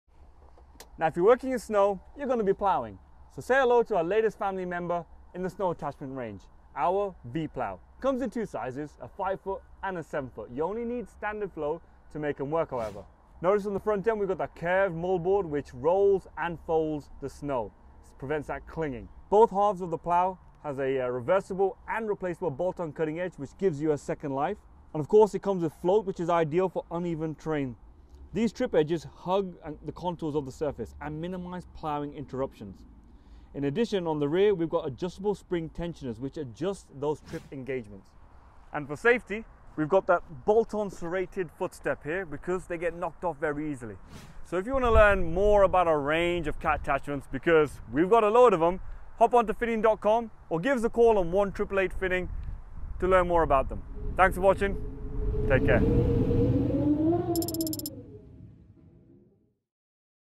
Cat® V-Plow
Cat® V-Plows Buckets for Cat Skid Steer Loaders,
Multi-Terrain Loaders, Compact Track Loaders, and Compact
Wheel Loaders are a versatile attachment for snow removal
applications with the capability to hydraulically angle five
different blade configurations. Push, plow, sweep, or scoop
snow and ice to clear surfaces and restore passage to
driveways, sidewalks, parking lots, and roadways.